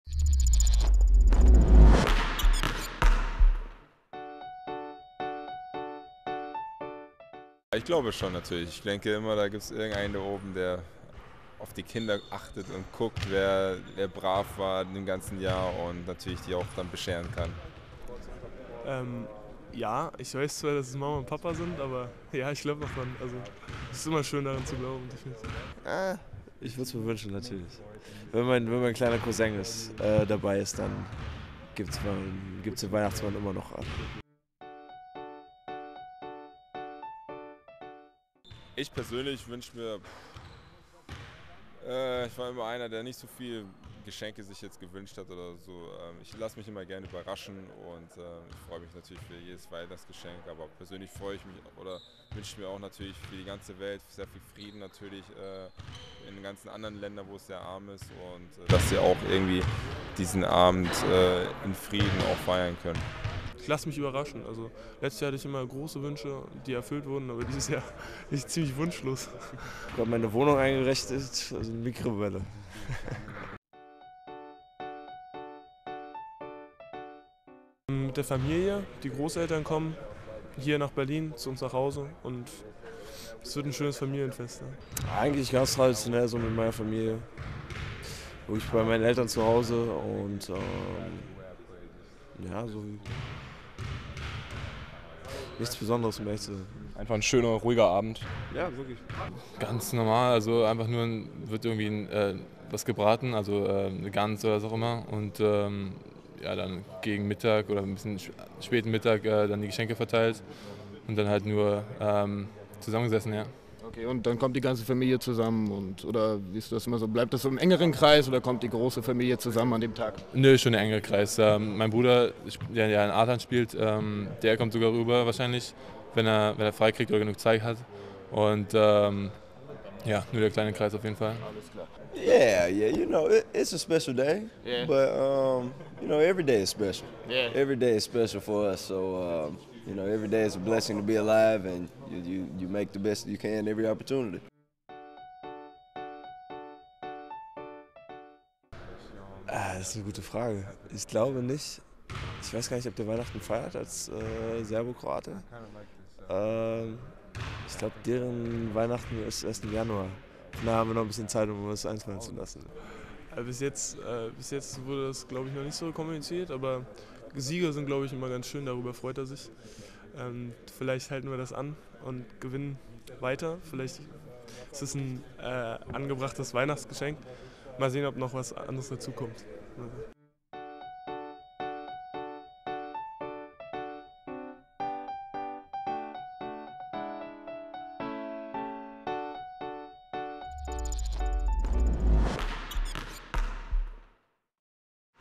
Die ALBA-Weihnachts-Umfrage
Gibt es den Weihnachtsmann? Was liegt beim Coach unterm Weihnachtsbaum? Die Studenten der Berliner Medienakademie fragten bei unseren Jungs nach.